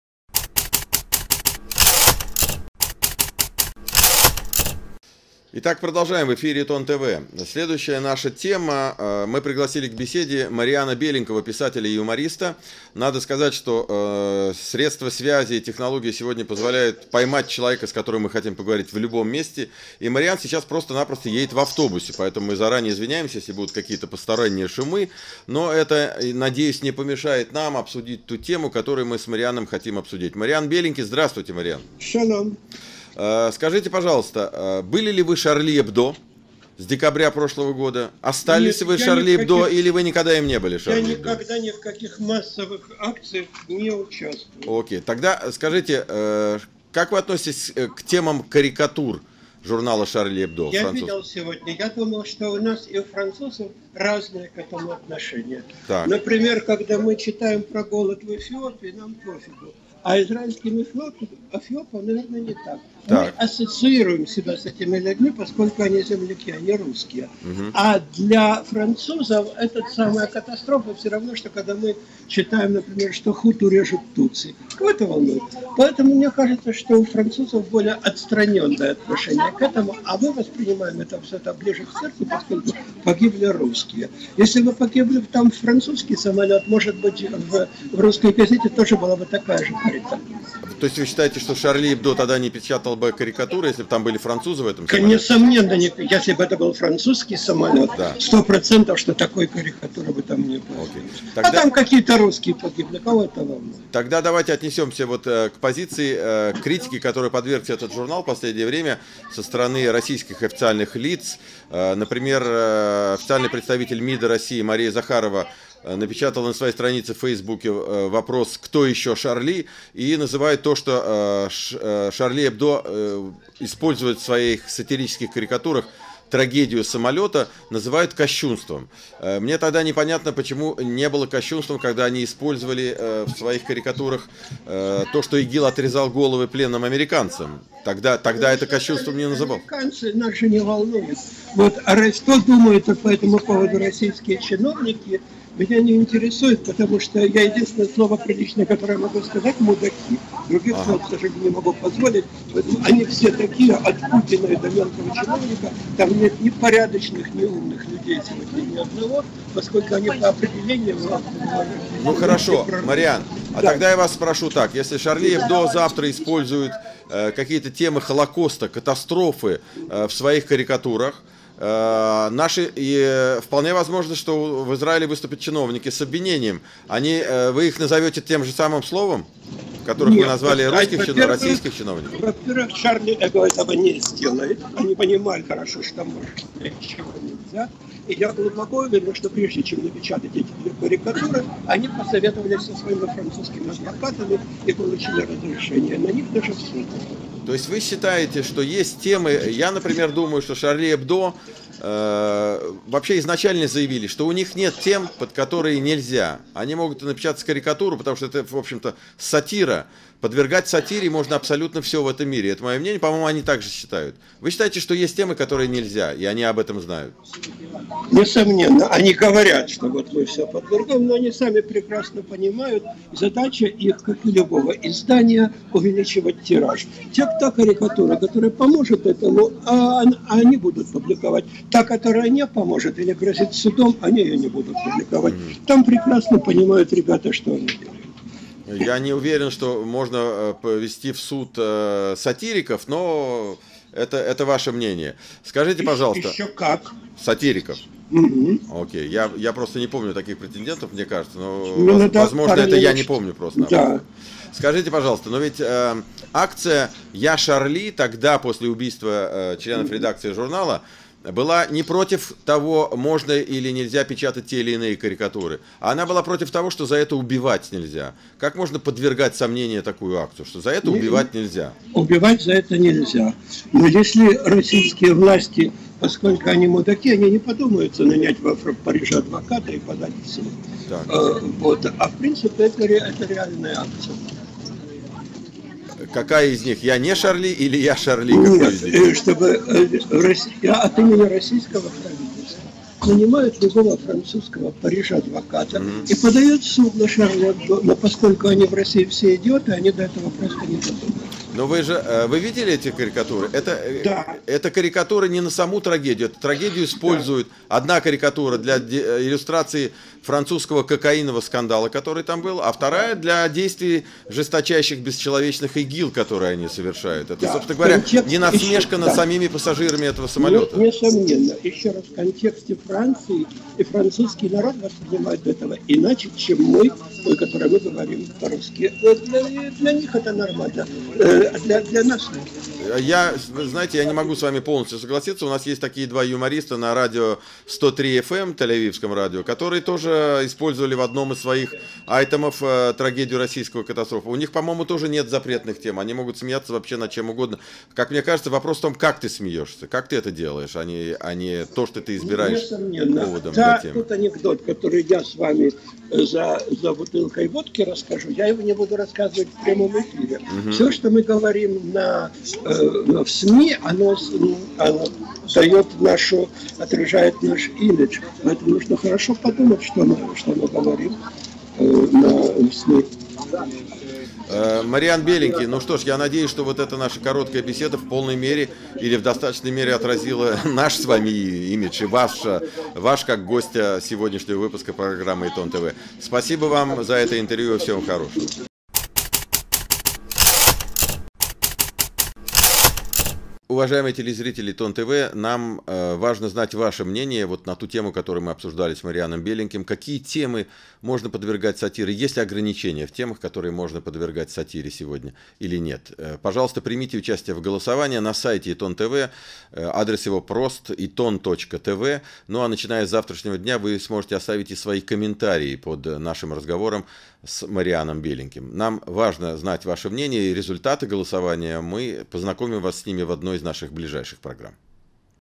"Шарли Эбдо": смеяться или мстить?
Карикатура француского журнала "Шарли Эбдо" на тему упавшего в Египте российского самолета вызвала в России волну протеста.
 Может ли карикатура быть возмутительной рассуждает сатирик Марьян Беленький.
Ведущий - Павел Маргулян